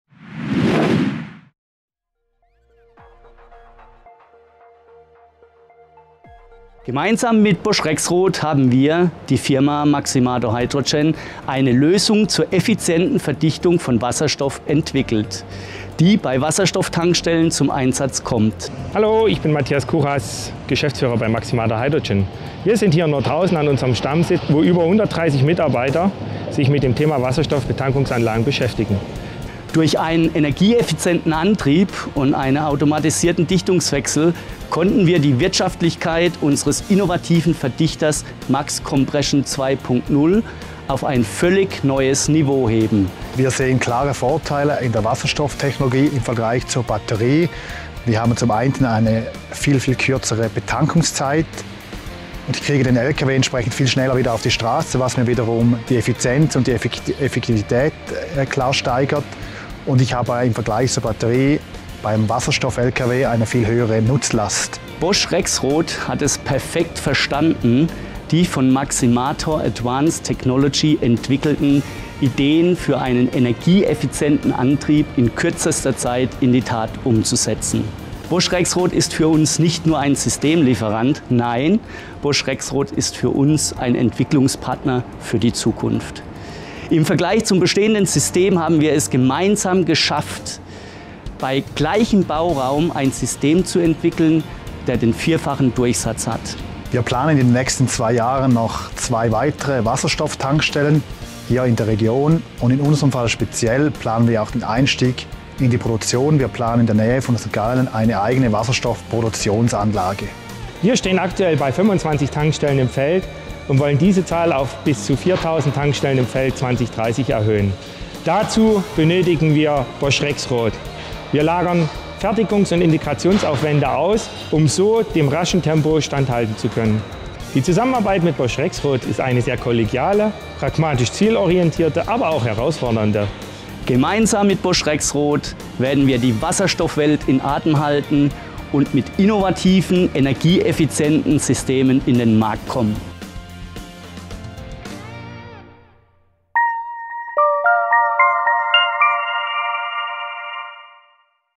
[DE] Bosch Rexroth & Maximator Hydrogen: Innovative Lösungen für Wasserstofftankstellen
Gemeinsam mit der Firma Maximator Hydrogen hat Bosch Rexroth eine Lösung zur effizienten Verdichtung von Wasserstoff entwickelt, die bei Wasserstoff-Tankstellen zum Einsatz kommt. Durch einen energieeffizienten Antrieb und einen automatisierten Dichtungswechsel konnte Maximator Hydrogen die Wirtschaftlichkeit ihres innovativen Verdichters Max Compression 2.0 auf ein völlig neues Niveau heben.
Durch den Einsatz digitaler Methoden, wie Virtual Engineering und Simulation, konnte die Engineeringphase und der Prototypenbau bedeutend beschleunigen werden. Mit Hilfe eines digitalen Zwillings und der virtuellen Inbetriebnahme lassen sich mögliche Fehler bereits vorab erkennen und beheben. Auch zukünftig wird der digitale Zwilling dabei unterstützen, das Produkt gemeinsam zu optimieren und weiterzuentwickeln.